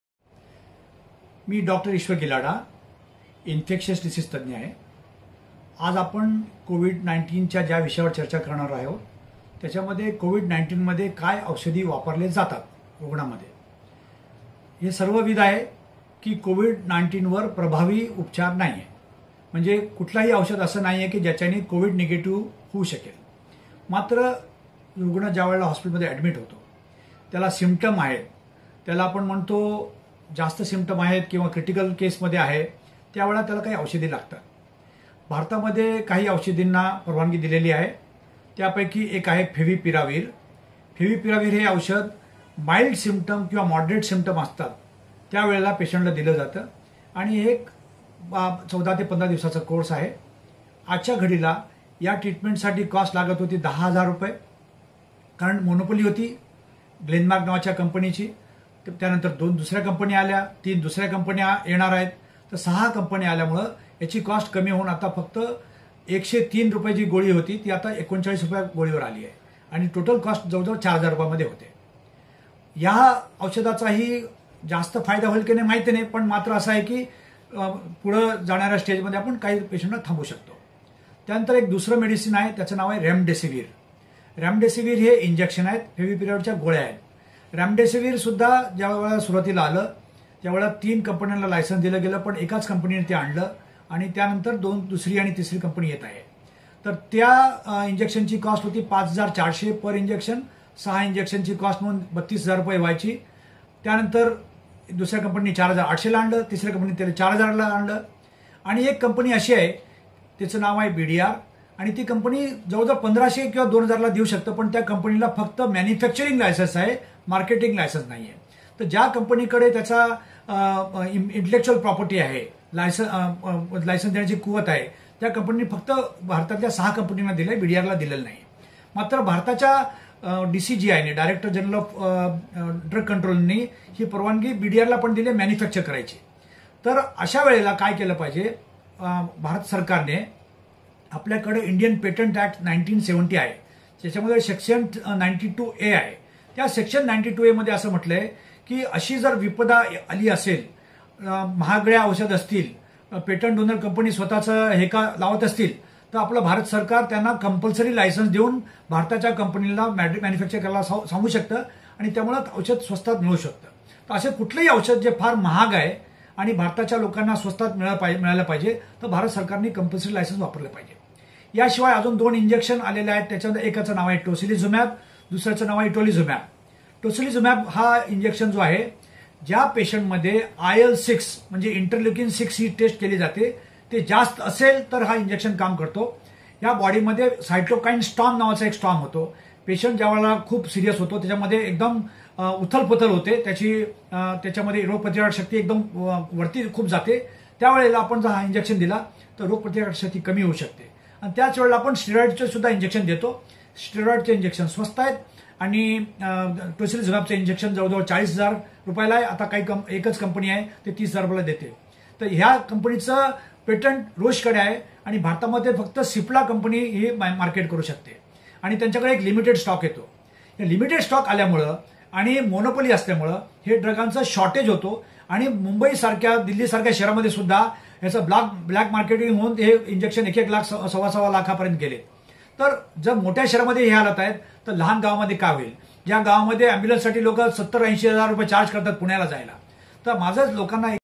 Dr Ishwar Gilada in Marathi on treatment of Symptomatic & Critical Covid 19 cases using newer drugs
Infectious diseases expert Dr Gilada discussing on treatment of mild, moderate Symptomatic cases as well as Critical Covid 19 cases using newer drugs like Favipiravir, Remdesivir, Tocilizumab and Itolizumab, their costs, monopoly, black marketing